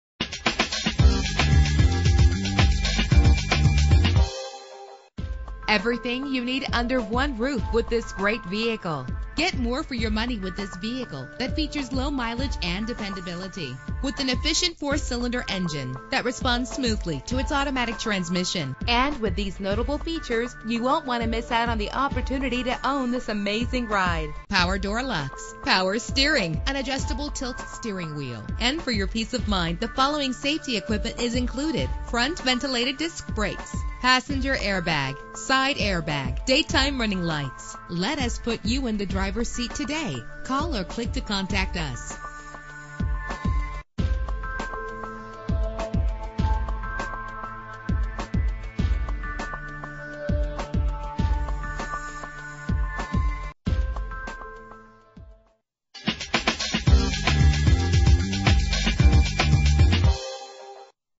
2010 Chevrolet Aveo5 - Kalispell MT
Year: 2010
Make: Chevrolet
Model: Aveo5
Trim: LT
Engine: 1.6 liter inline 4 cylinder 16 valve
Transmission: 4-Speed Automatic
Color: Cosmic Silver
Mileage: 2147
Address:
2505 Highway 2 East